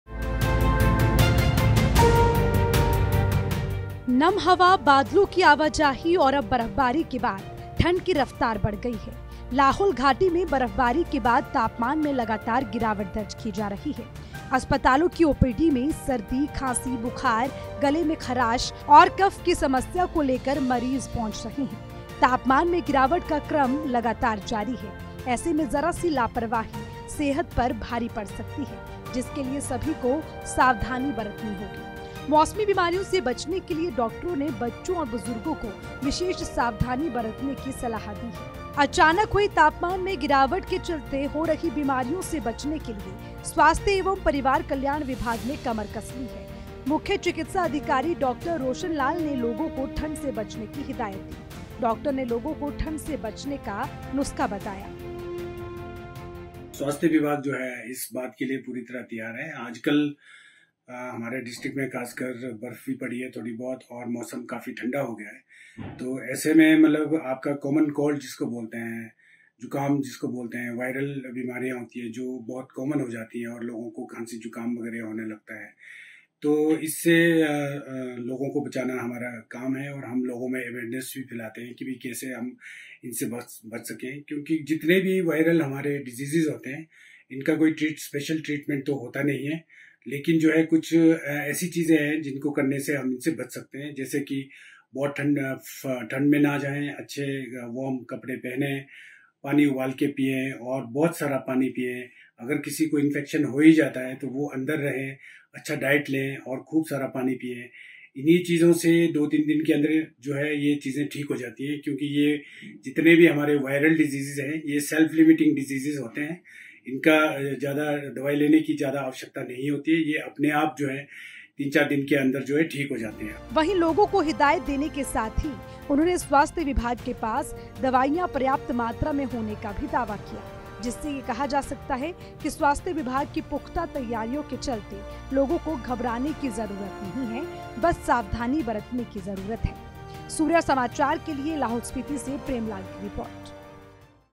Himachal में मौसम का बदला मिजाज, मौसमी बीमारी का खतरा बढ़ा
हिमाचल प्रदेश में मौसम ने करवट ली है जिसके चलते ठंड भी बढ़ गई है... जिला लाहौल-स्पीति की ऊंची चोटियों पर तापमान में गिरावट दर्ज की गई है..और अब बढ़ती ठंड से लोगों को मौसमी बीमारियों का डर सताने लगा है...इस बीच राहत की बात ये है कि प्रशासन की तैयारी पुख्ता है. देखिए पूरी खबर .